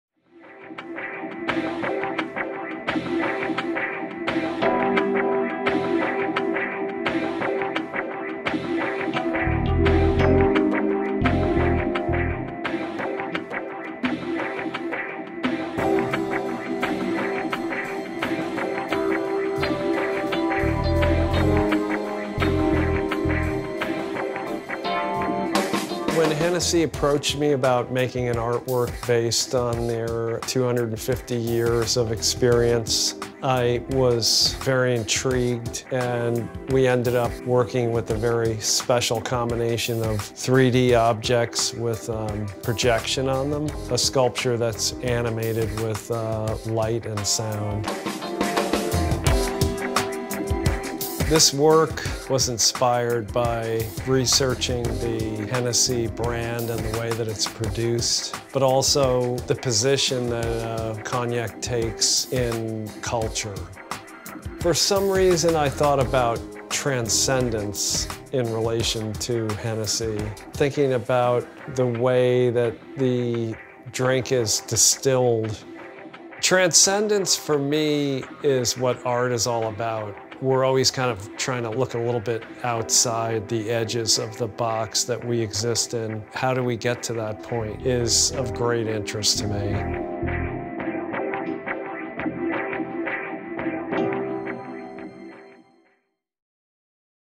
Hennessy 250 Tour - Tony Oursler
Hennessy 250 Tour - Tony Oursler. Behind the scenes with artist Tony Oursler to discover his artwork for the Hennessy 250 Tour.

The Hennessy family were early patrons of the arts and have continued this tradition with artists and designers of the 21st century. A range of artists have contributed works to the H250 Tour, a global traveling exhibiton that defines the spirit of Hennessy.